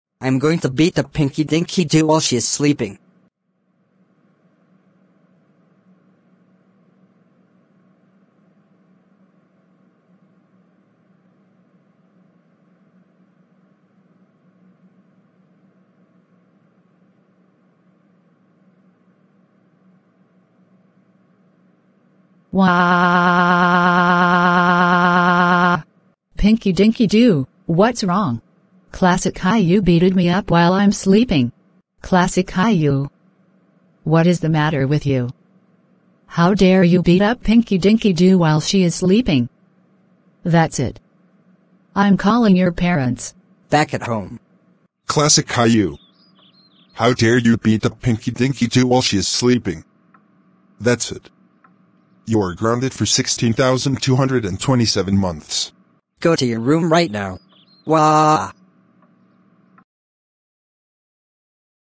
Classic Caillou Beats Up Pinky Dinky Doo While She Is Sleeping And Gets Grounded
Red Cinema 2